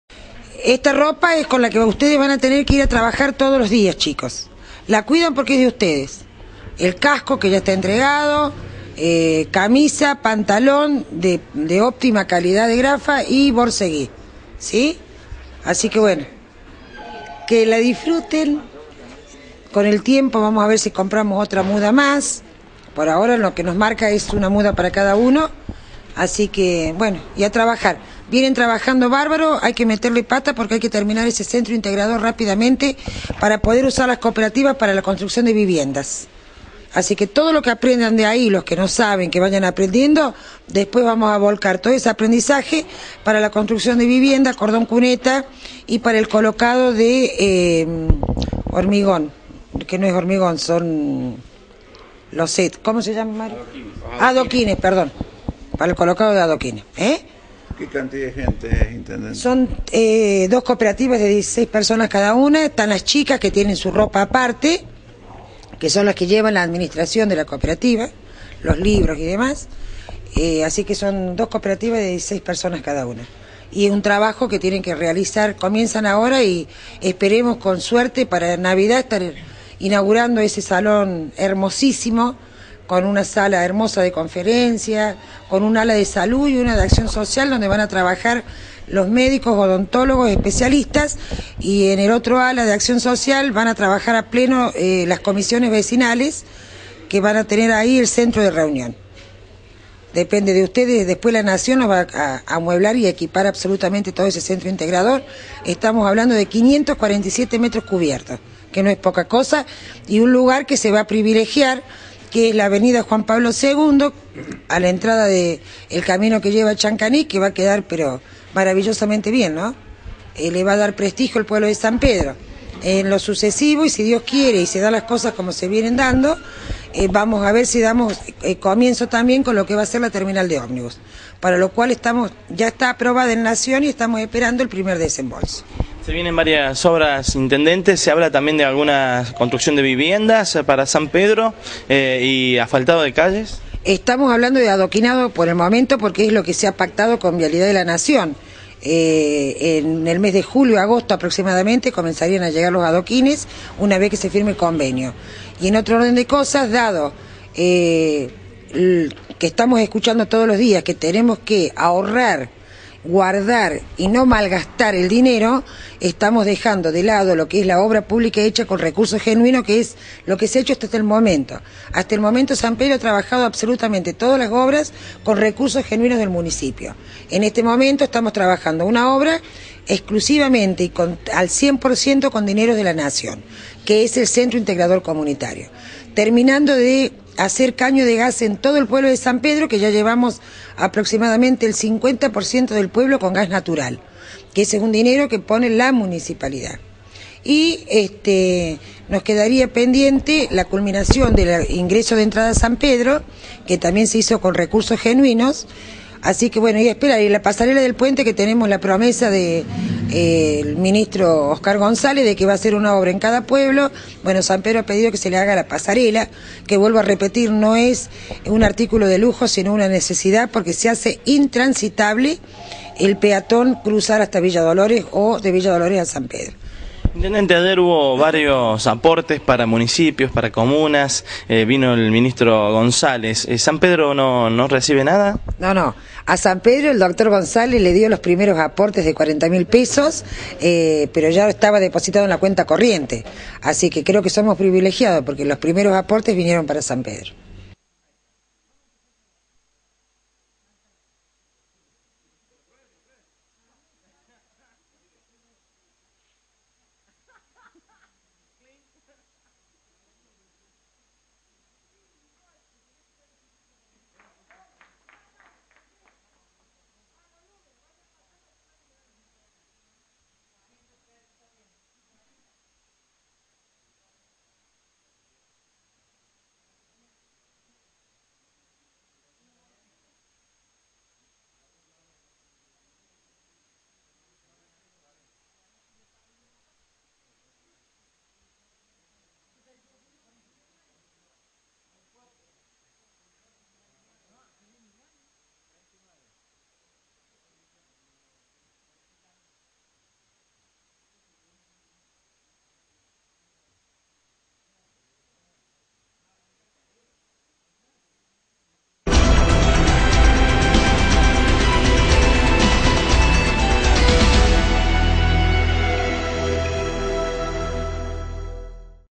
ENTREGA DE INDUMENTARIA PARA COOPERATIVAS DE TRABAJO
En las instalaciones del Municipio de San Pedro la Intendente Emilia Calvo procedió a entregar la indumentaria de trabajo a quienes integran las dos cooperativas responsables de la construcción del CIC (Centro integrador Comunitario). El equipamiento cuenta con borceguíes, camisa y pantalones.
Esto nos decía la mandataria municipal.